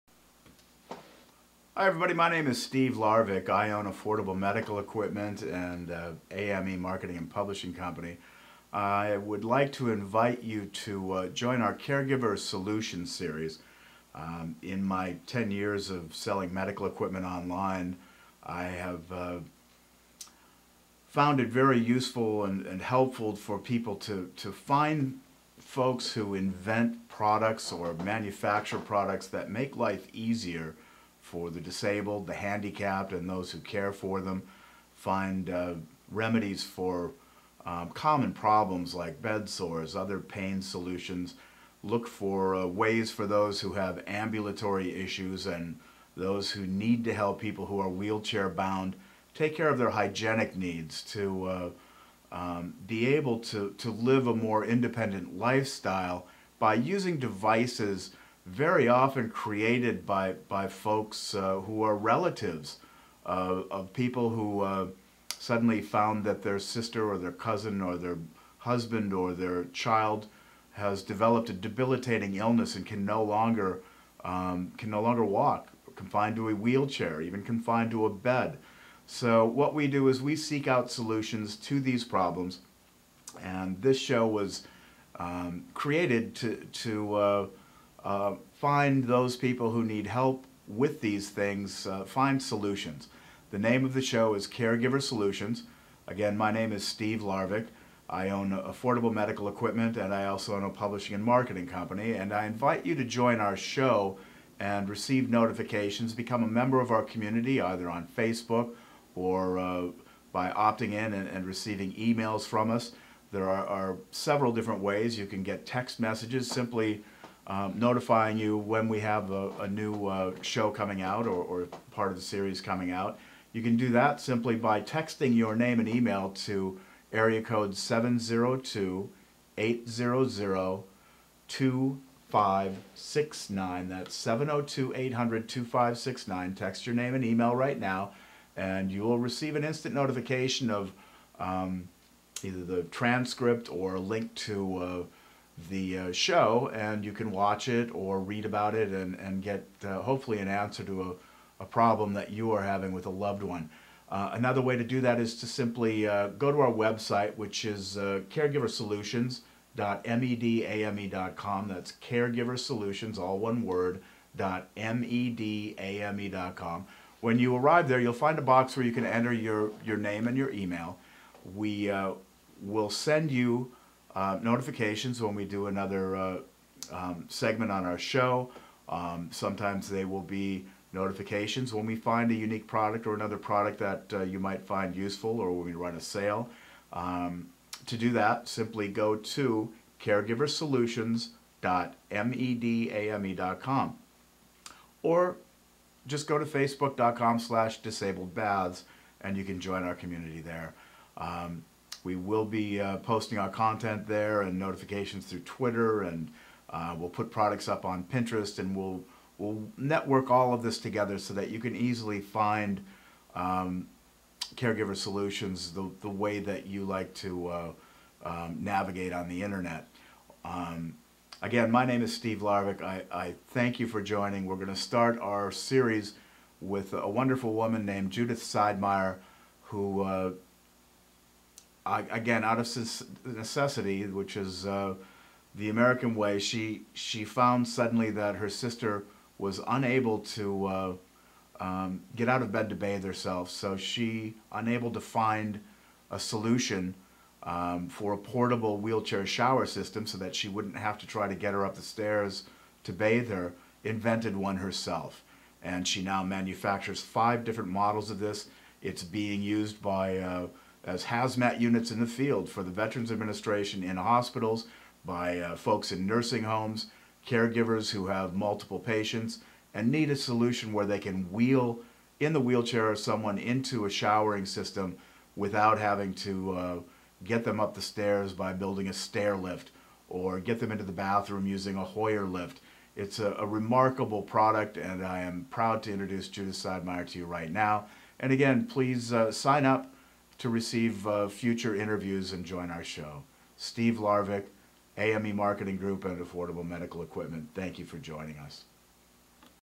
Welcome to Caregiver Solutions - Affordable Medical Equipment
Welcome to Caregiver Solutions. Affordable Medical Equipment seeks out the inventors and manufacturers of solutions to unique problems for the disabled, handicapped and those who care for them. or text your name and email to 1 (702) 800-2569 to be notified and receive users guides and instructions for use of products we promote on the show.